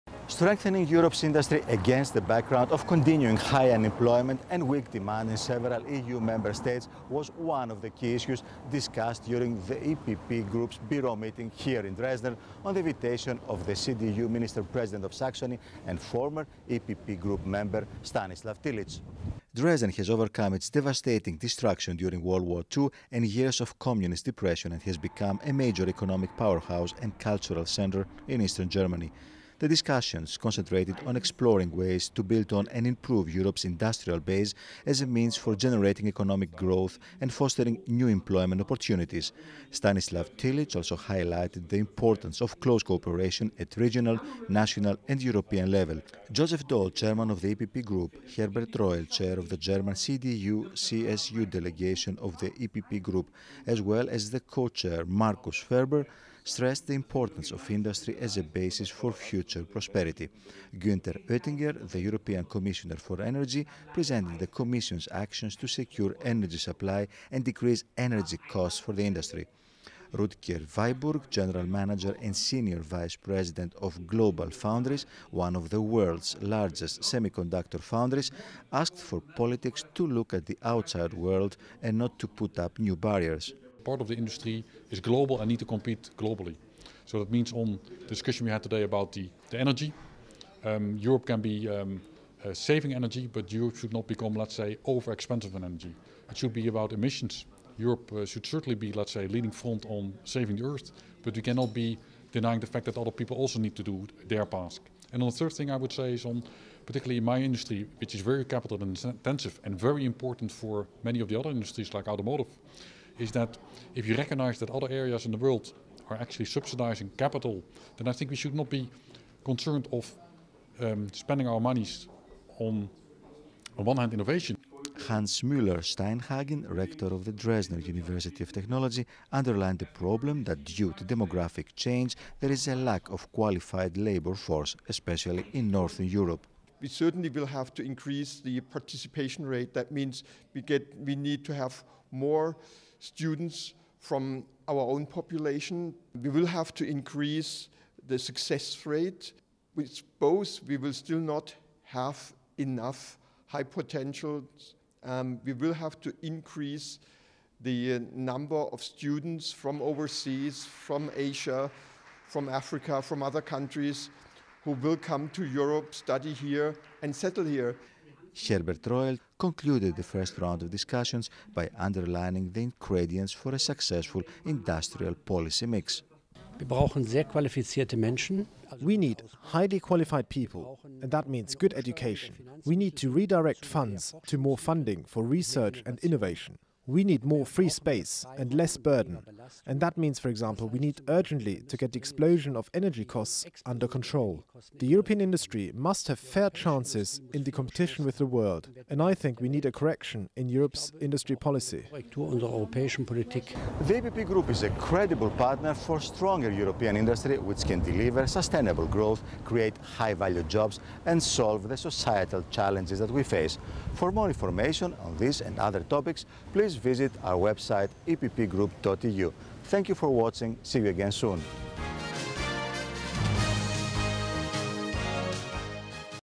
The Importance of Industry to Generate Growth in Europe
The Bureau of the EPP Group in the European Parliament held two-day political talks in Dresden, Germany and discussed ways to strengthen Europe's industry against the background of continuing high unemployment and weak demand in several EU Member States. The discussions on the invitation of the CDU Minister-President of Saxony and former EPP Group Member, Stanislaw Tillich, concentrated on exploring ways to build on and improve Europe's .